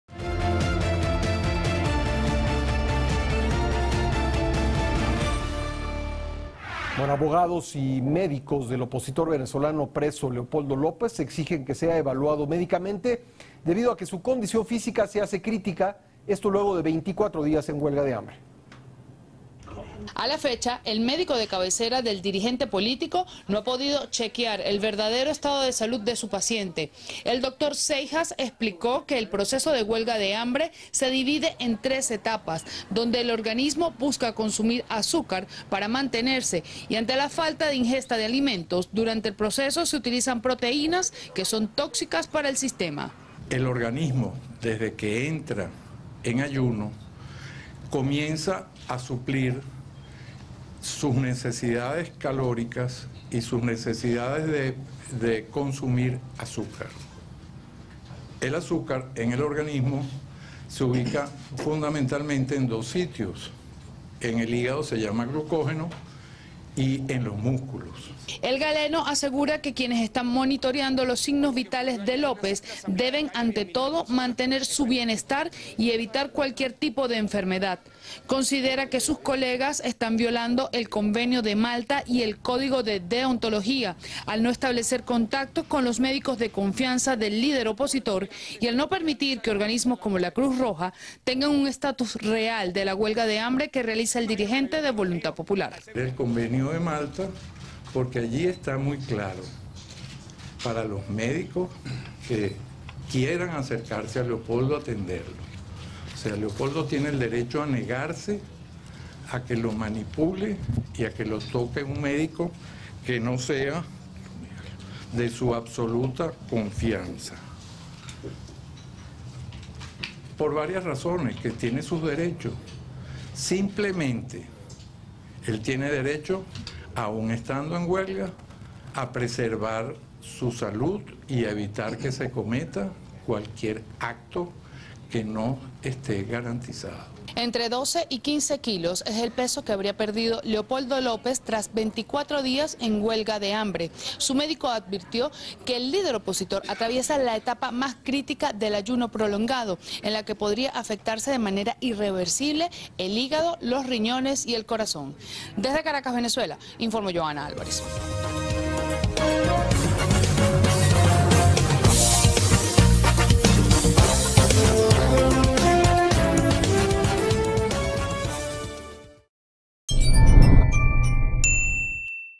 En Venezuela exigen valoración médica de Leopoldo López / Titulares de la Noche
El Líder opositor venezolano, Leopoldo López , Continúa en huelga de hambre y doctores exigen una valoración médica de su condición.

18 de junio 2015

ISIS vende a mujeres por el precio de una cajetilla de cigarros